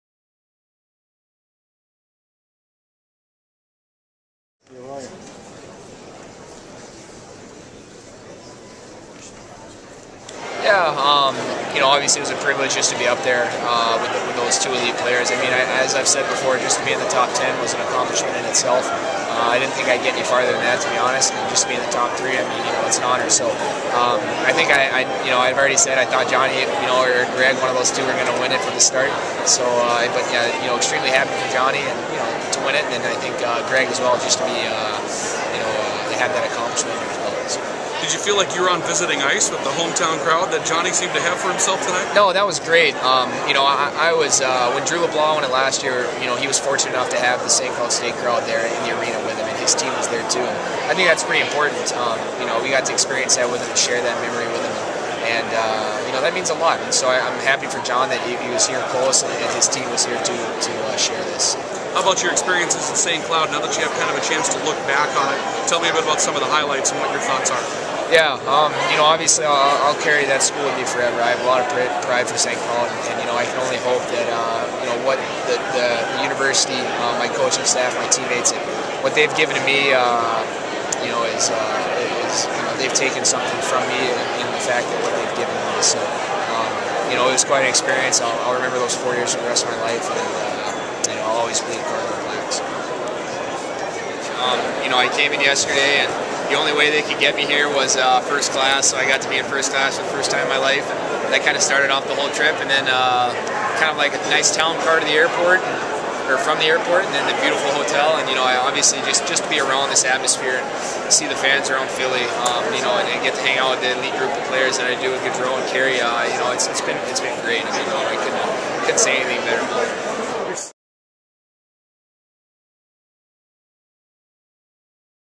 St. Cloud State' Nic Dowd at Hobey Ceremony on April 11, 2014
St. Cloud State's Nic Dowd at 2014 Hobey Baker Award Ceremony on April 11, 2014 in Philadelphia, Pa.